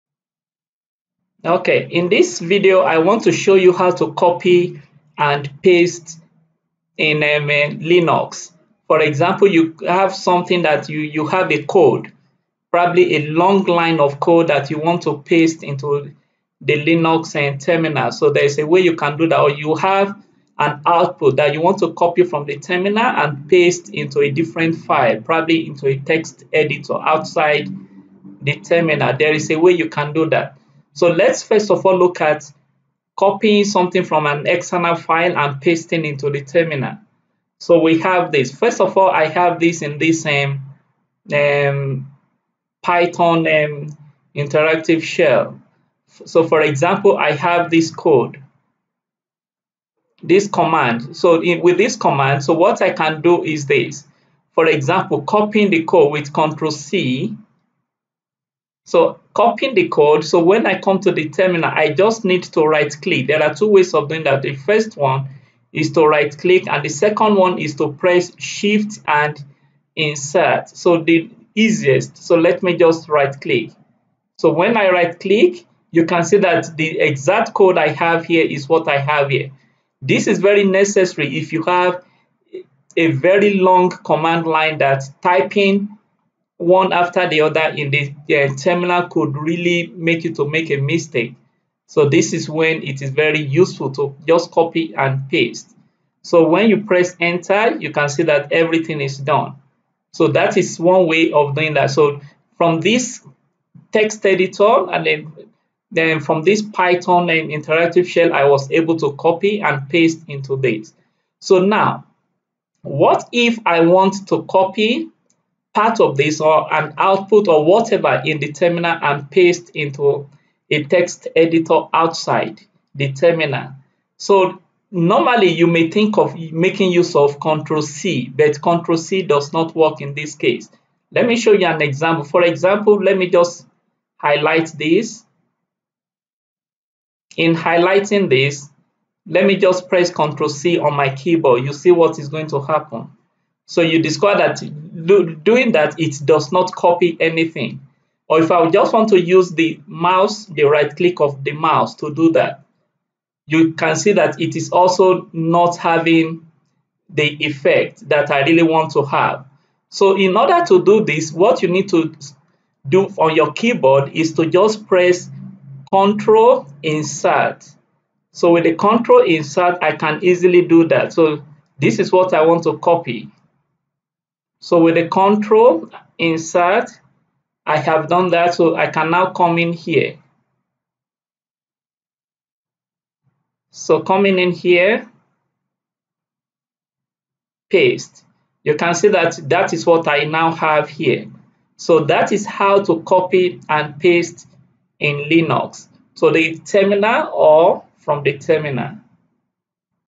How to Copy and Paste Command or Output in Linux
Your usual Ctrl + C keyboard shortcut does not work here in Linux. This video shows how to Copy and Paste Command or Output in Linux with certain keyboard shortcuts and the mouse